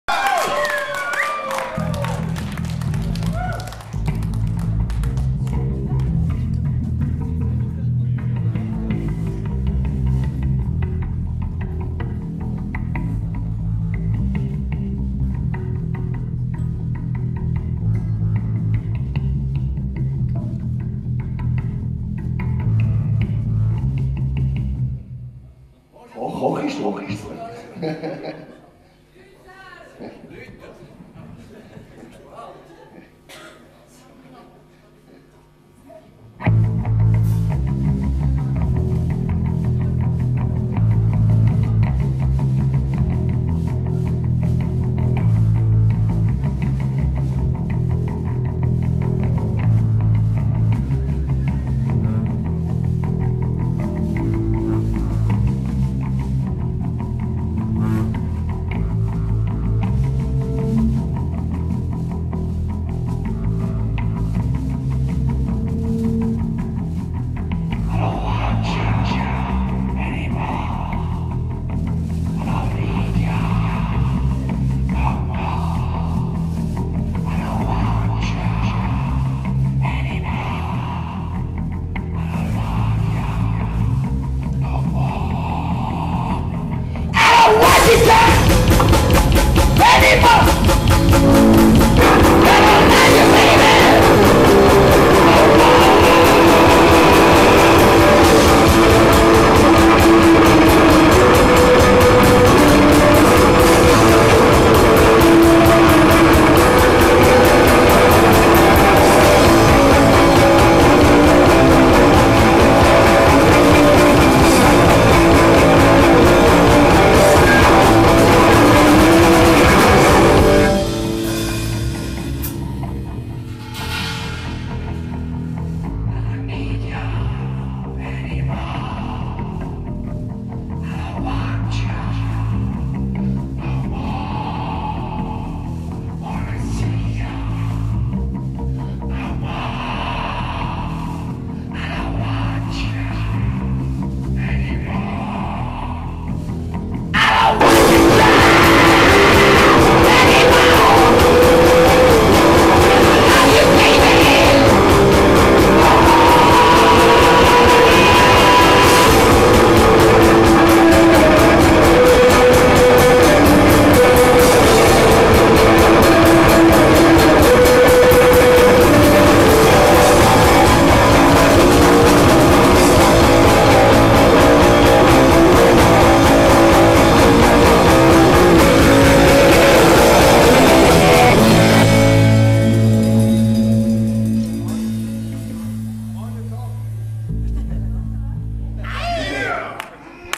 The Monsters au MILF Festival
The Monsters au MILF Festival (Kremlin, Monthey, VS), le 24.02.18, filmé par Olive